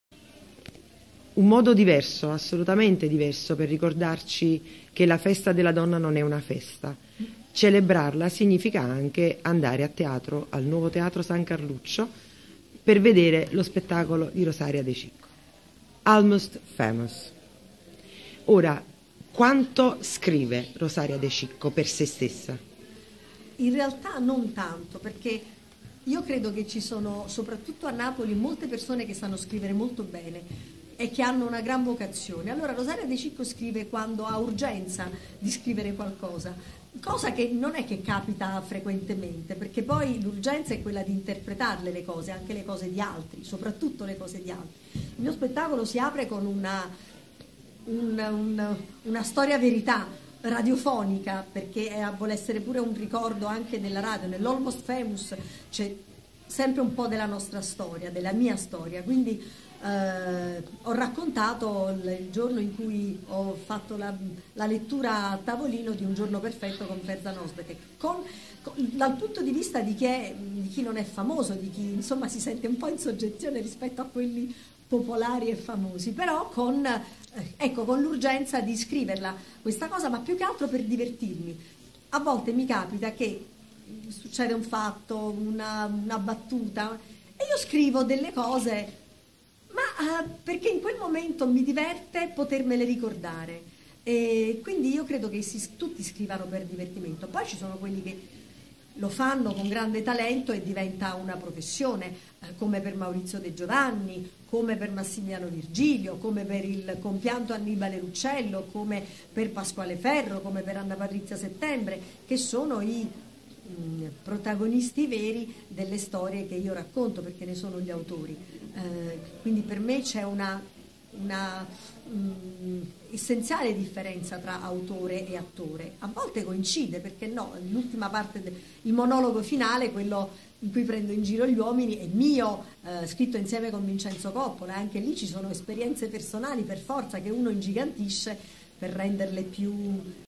Intervista a Rosaria De Cicco
Monica Lucignano intervista Rosaria De Cicco per Magazine Pragma